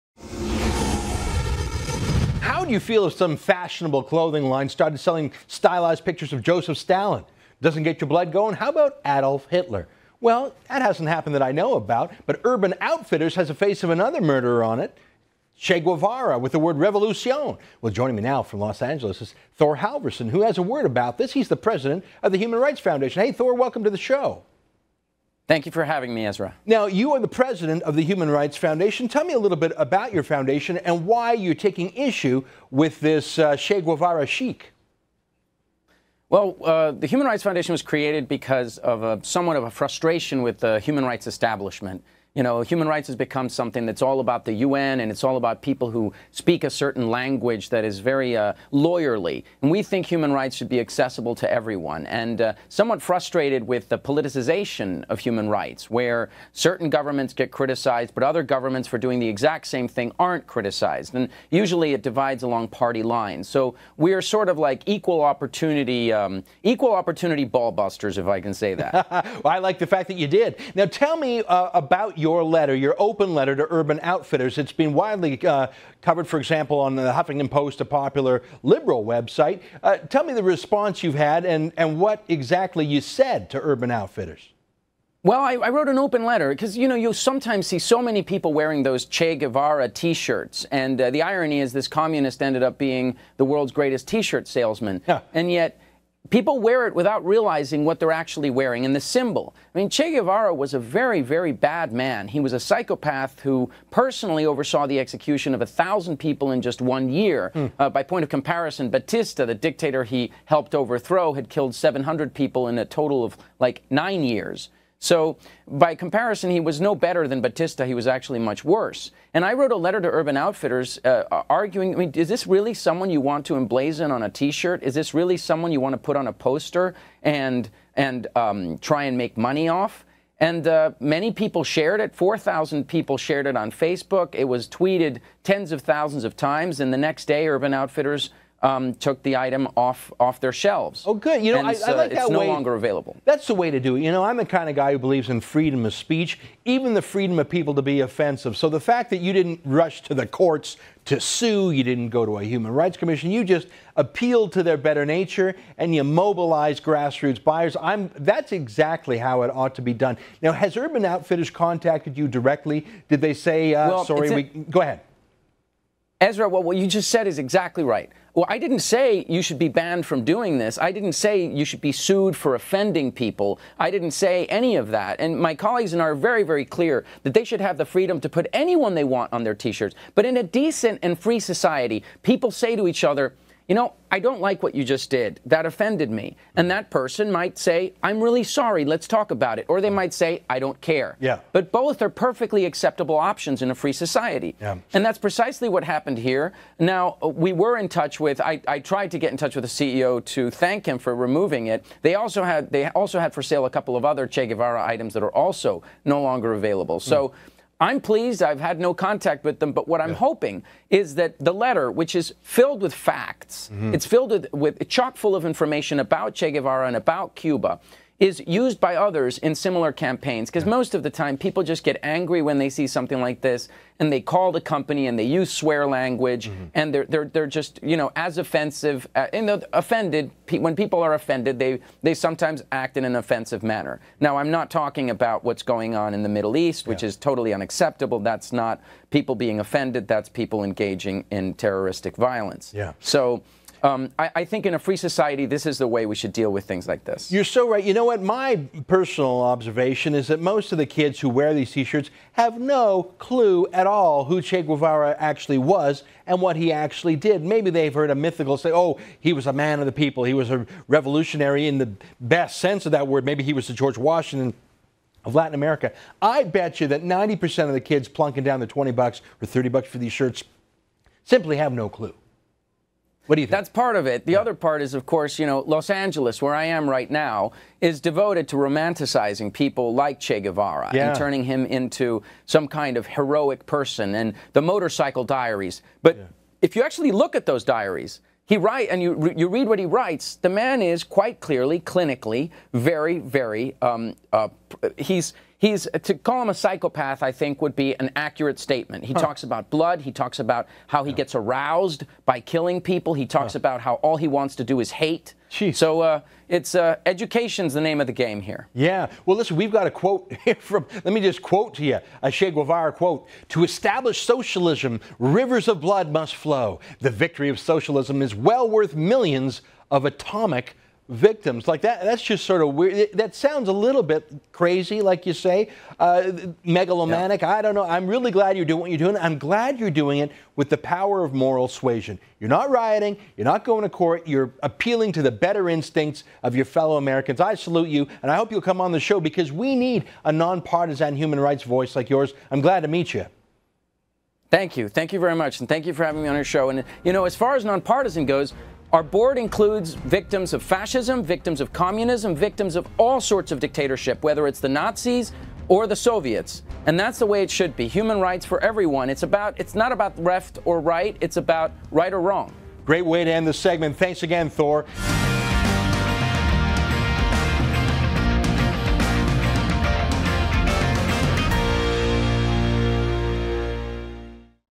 Che Guevera's not chic
Thor Halvorssen on his open letter to Urban Outfitters to stop selling t-shirts that idolize Che Guevara.